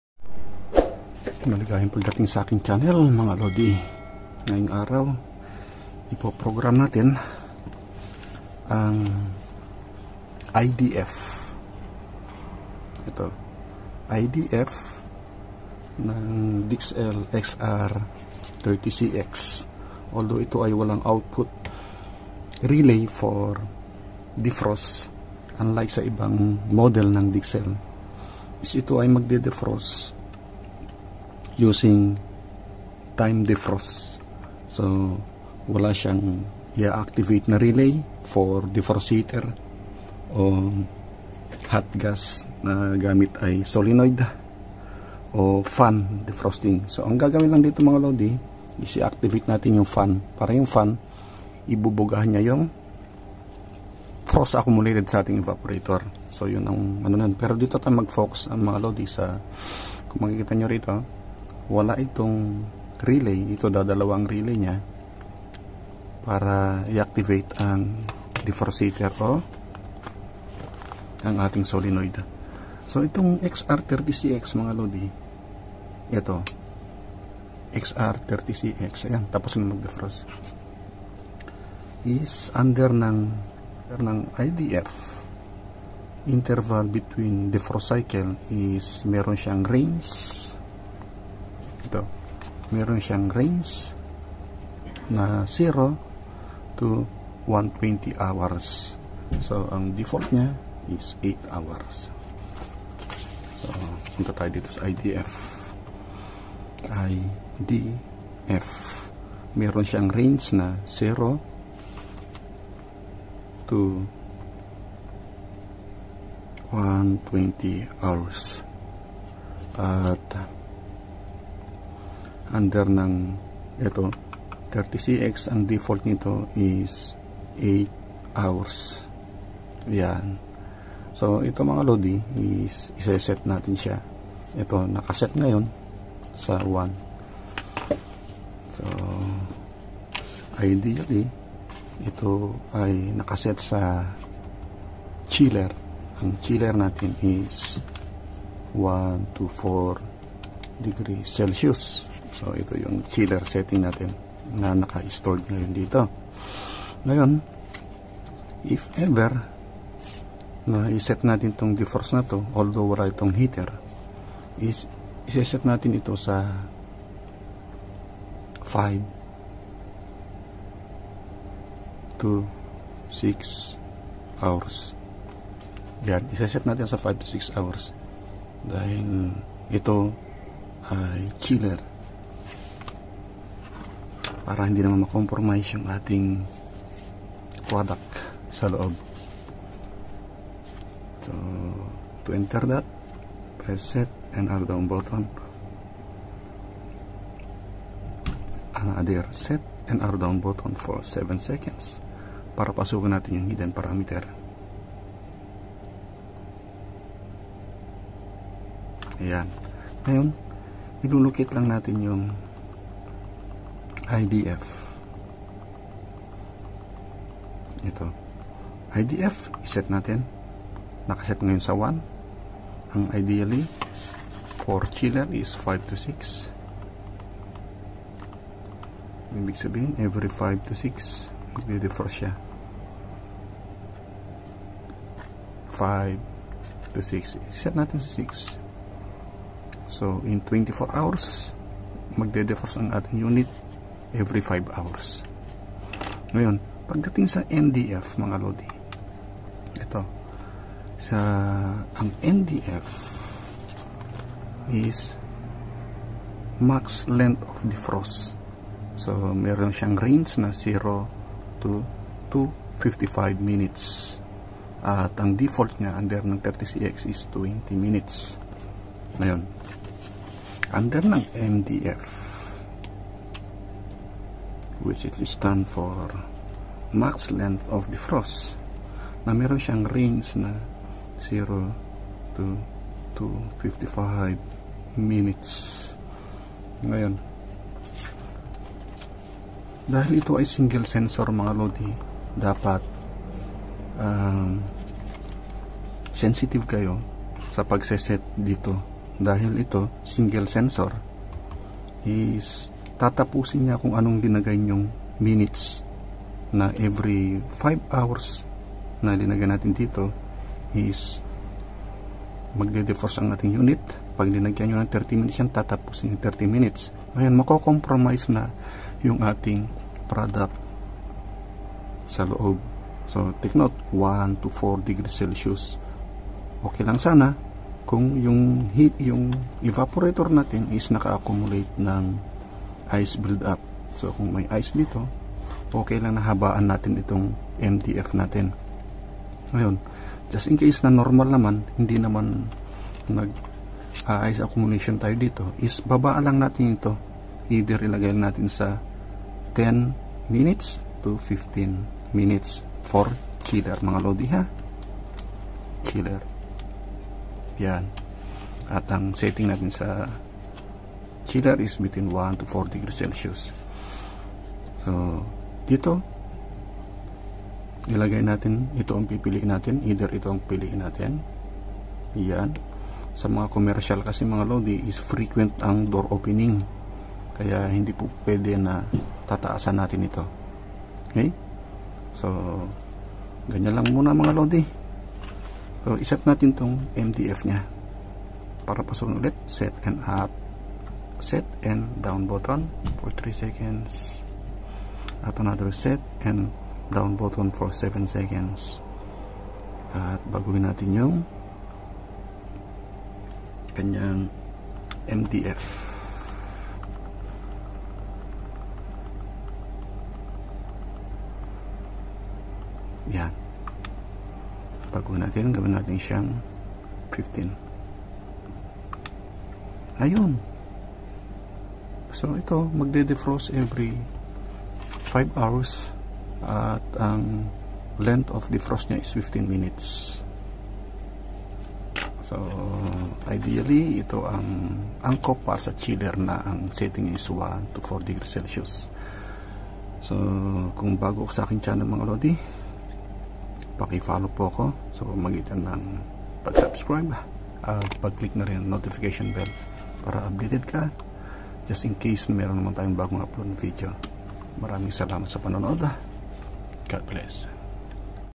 DIXELL XR30CX DEFROST INTERVAL AND MAX LENGTH OF DEFROST SETTINGS FOR CHILLER 1°C TO 4°C
Chiller's parameters of IDF and MDF for dixell xr30cx, a rectification of my IDF settings in my previous programming.

*DixellDefrostSettings
*DixellXr30cx
*OCTTV